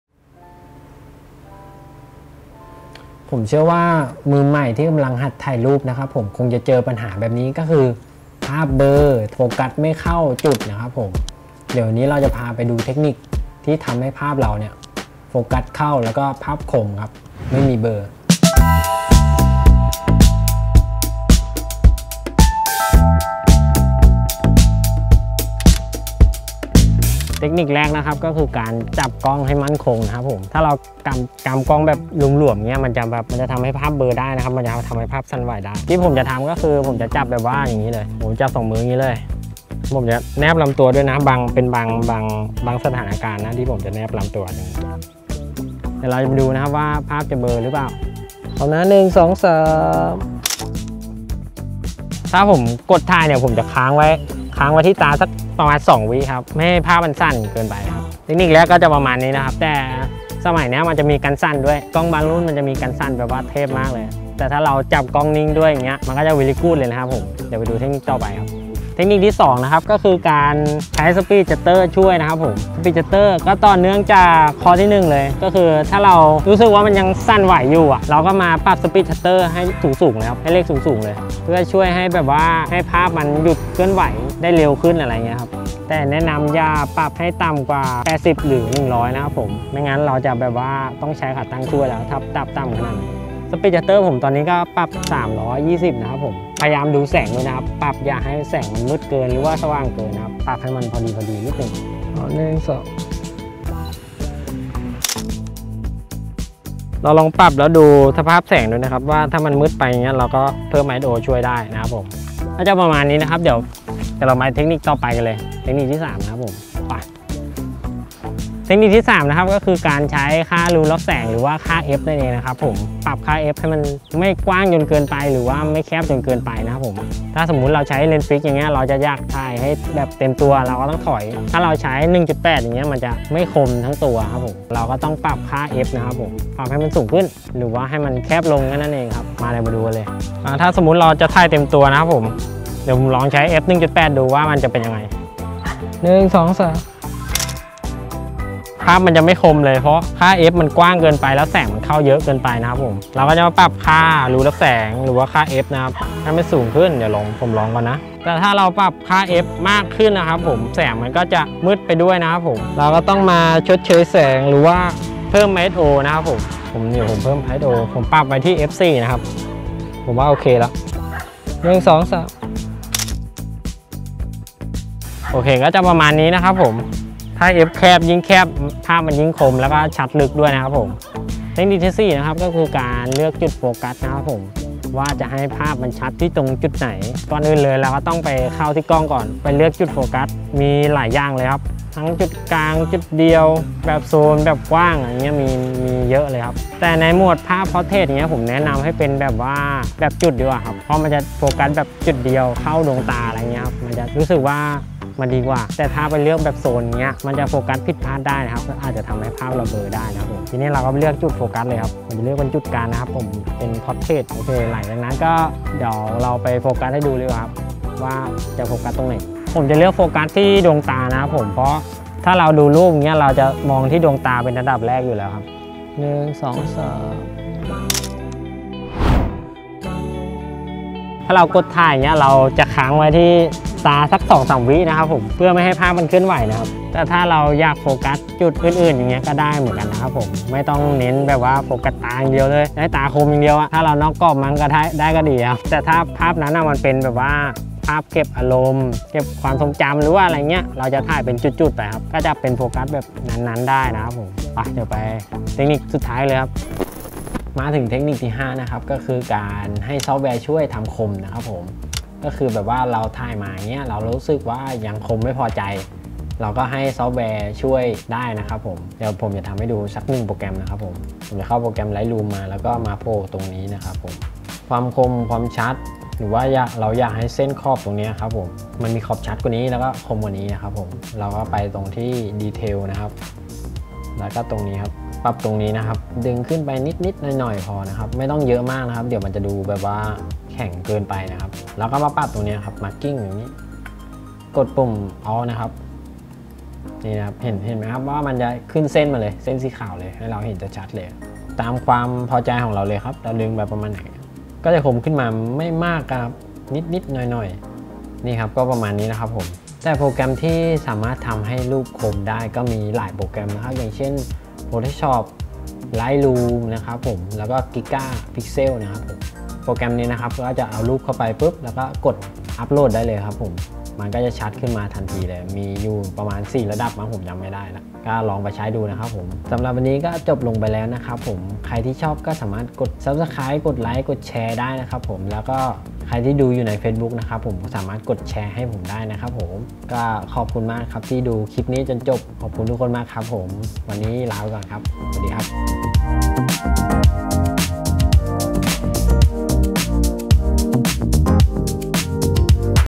5 เทคนิคถ่ายภาพไม่มีเบลอ (มือใหม่ห้ามพลาด)
เทคนิคนี้จะเป็นสำหรับมือใหม่นะครับ ถ้ามีอะไรผิดพลาดต้องขออภัยด้วยนะครับผม

𝘓𝘦𝘯𝘴 : 50 𝘮𝘮 𝘧1.8

𝘗𝘩𝘰𝘵𝘰𝘨𝘳𝘢𝘱𝘩𝘦𝘳 : 𝘗𝘪𝘵𝘪𝘱𝘰𝘯𝘨 𝘚𝘦𝘢𝘭𝘢𝘭𝘢𝘬
𝘔𝘰𝘥𝘦𝘭 : 𝘤𝘩𝘰𝘴𝘪𝘵𝘢 𝘕𝘶𝘯 ( 𝘪𝘨: nunchosita )
𝘓𝘰𝘤𝘢𝘵𝘪𝘰𝘯 : บ้านเราเอง

ติดตามผลงานภาพถ่ายกันได้ที่

𝐅𝐨𝐫 𝐰𝐨𝐫𝐤:
𝐋𝐢𝐧𝐞: pitipong.